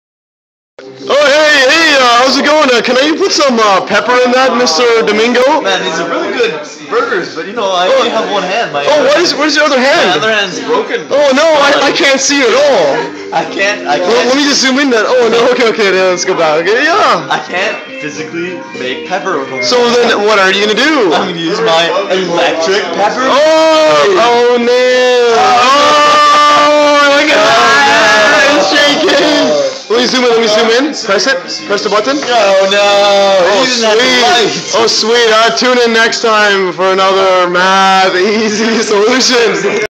Amazing Electric Pepper Shaker
Domingo Razmirez injured one of his arms so he can't use a manual salt shaker as it requires 2 hands. But with this amazingly new Electric Pepper Shaker, he can shake like there is no tomorrow ;)